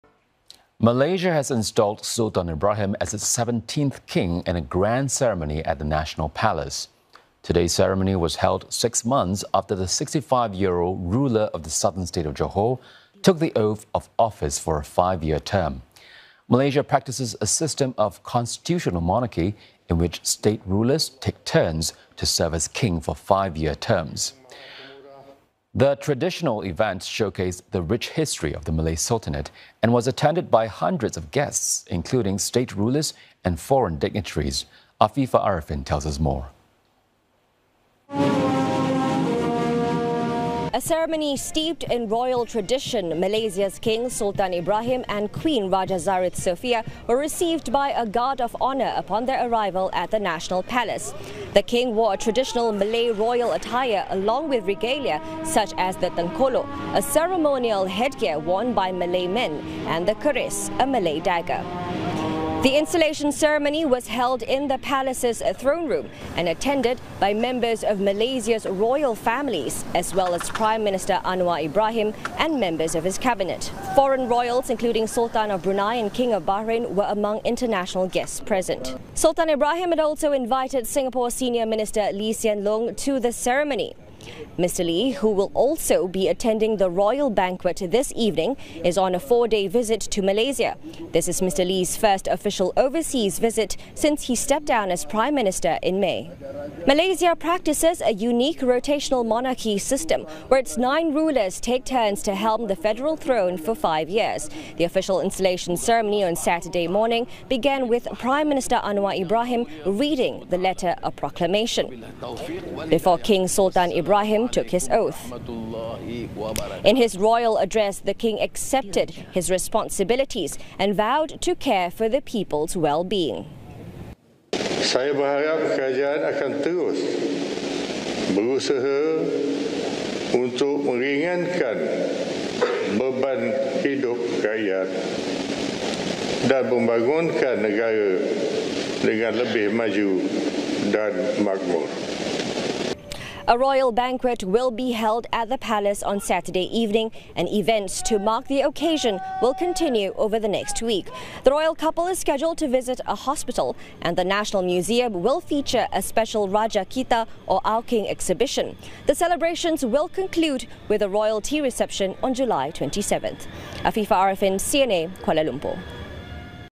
Sultan Ibrahim officially installed as Malaysia’s 17th king
Malaysia has installed Sultan Ibrahim from Johor as its 17th king, in a grand ceremony at the National Palace in Kuala Lumpur. The traditional event attended by hundreds of guests showcased the rich history of the Malay sultanate. Afifah Ariffin tells us more.